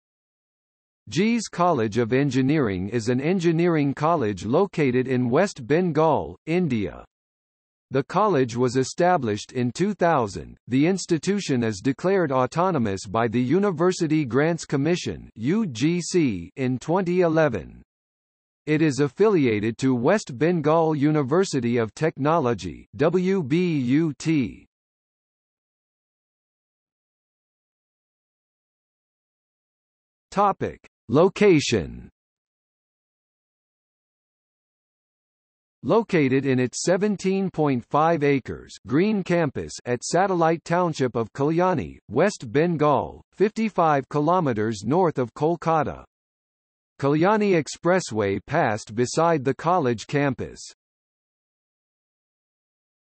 JIS College of Engineering | Wikipedia audio article
00:00:26 Location

- Socrates

SUMMARY
JIS College of Engineering is an engineering college located in West Bengal, India. The college was established in 2000.The Institution is declared Autonomous by the University Grants Commission (UGC) in 2011.